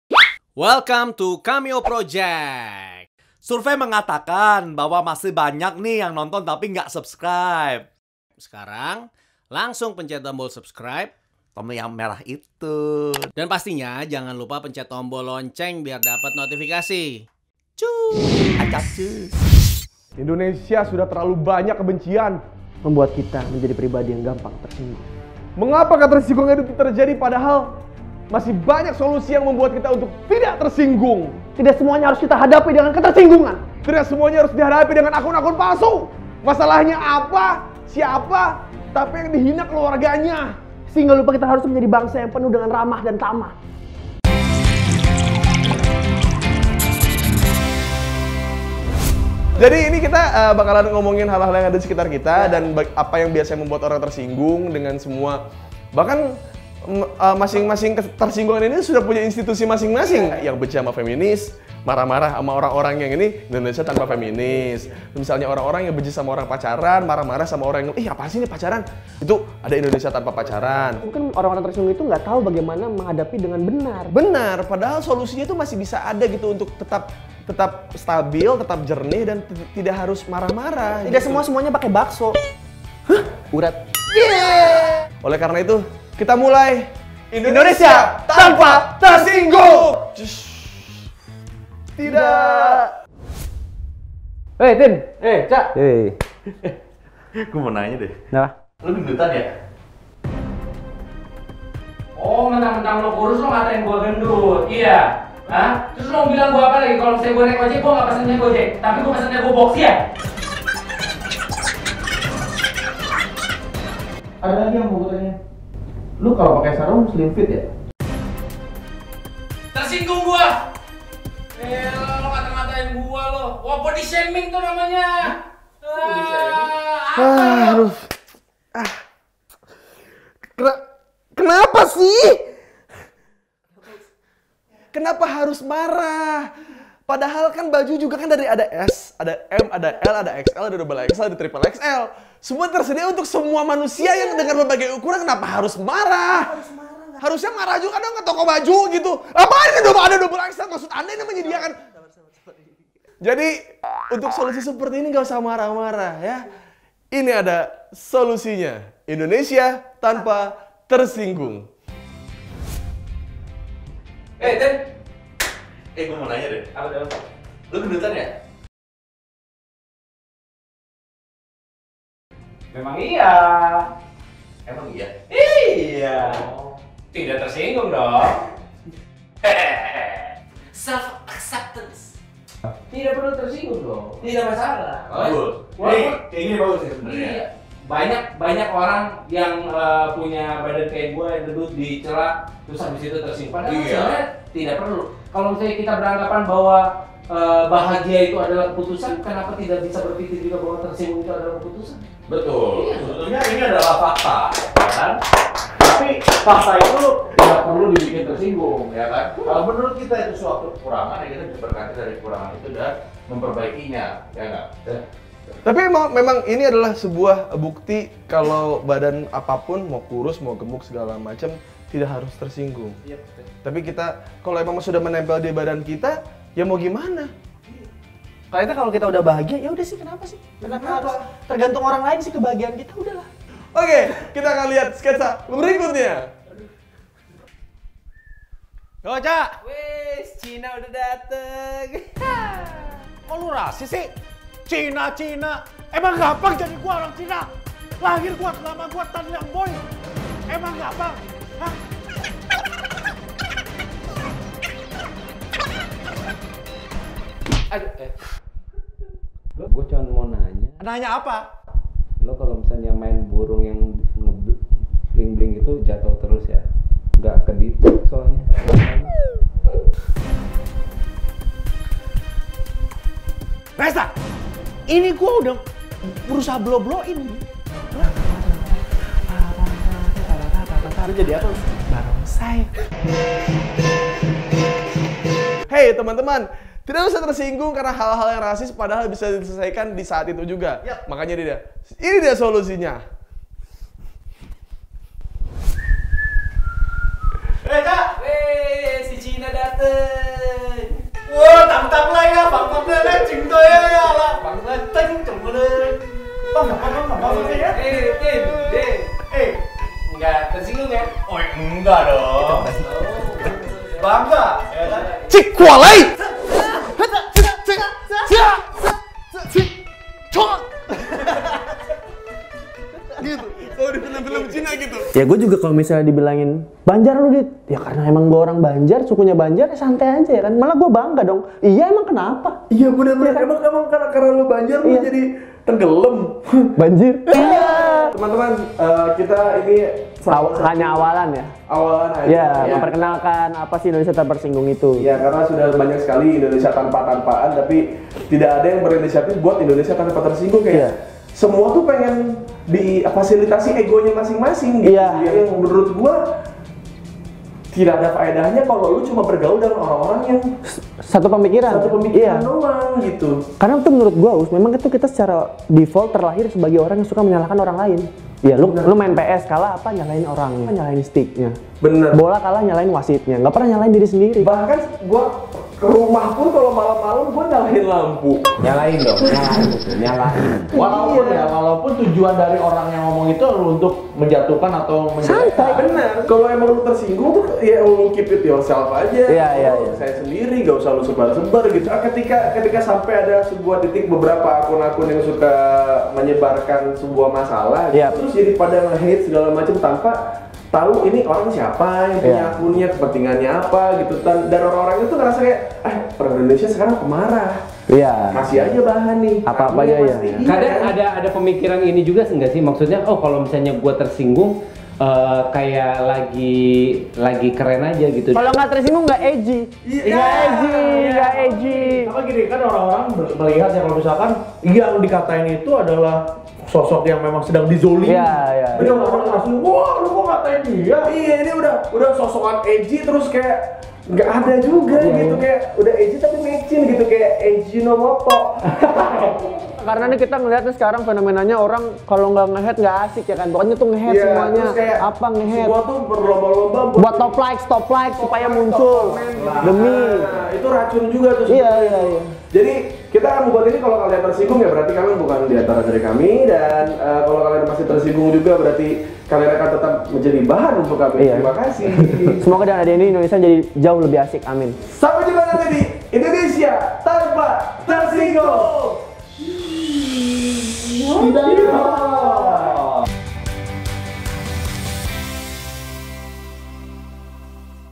INDONESIA TANPA TERSINGGUNG (UUS+ARDIT)
Kenapa harus marah? kenapa harus baper? kenapa harus tersinggung? kalau itu faktanya, sans aja!!

Thanks to :
Ardit Erwandha
UUS